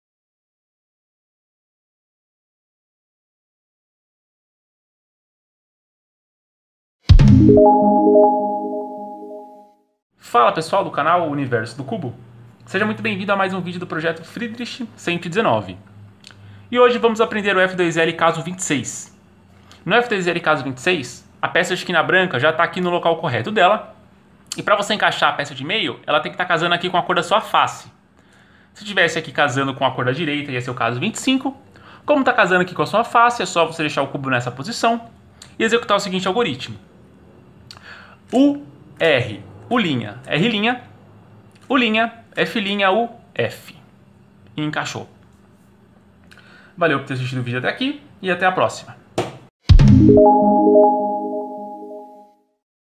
Projeto Fridrich 119 - Vídeo 026/119 - F2L Caso 26
00:00 - Introdução
00:10 - F2L caso 26: U R U’ R’ U’ F’ U F
00:42 - Finalização

Os links úteis para compra de livros, download de prévias e playlists de vídeos estão todos organizados nos linktrees abaixo.